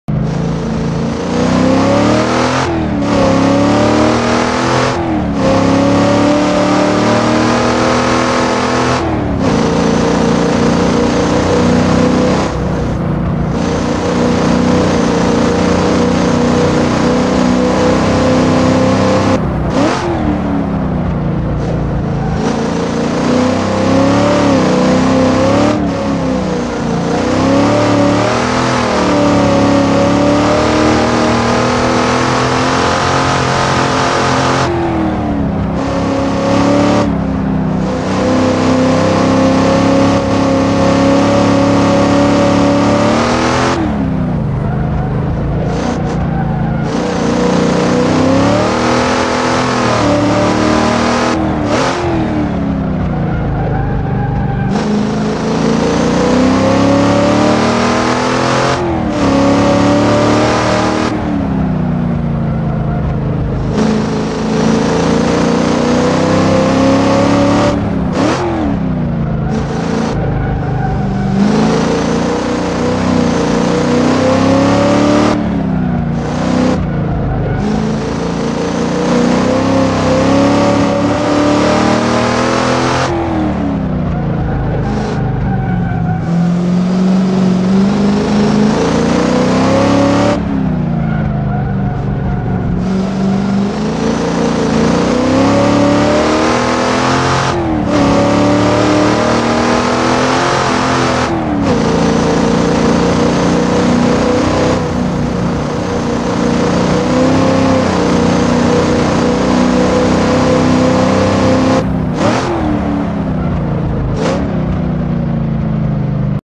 Ahvenisto Circuit 60's/70's Time Attack Entry #5: 1966 Chevy Nova SS 350 Block | 1:32.407
A great mod from Uncle M, we finally get to the 1st American car of this Time Attack series, and it's the 1966 Chevy Nova SS with the 350 block in it. The mid-end torque was constantly at battle with the modernized Corvette C7 semi-slick rubber I picked for the car to run on, but boy did it make a difference. It was a fun challenge too.

I went 1:32 flat on one lap, but it wasn't clean enough to record for this series. But still, to be within a second of the Ferrari 250 GTO is quite a feat, I wasn't expecting this car to be able go into the 1:32's.

default abs

Weather and track conditions:

-Weather: Windy(Sol)
-Ambient temperature: 10 degrees celsius
-Track temperature: 10 degrees celsius
-Wind: 16-19 km/hr, going ENE
-Track grip 100%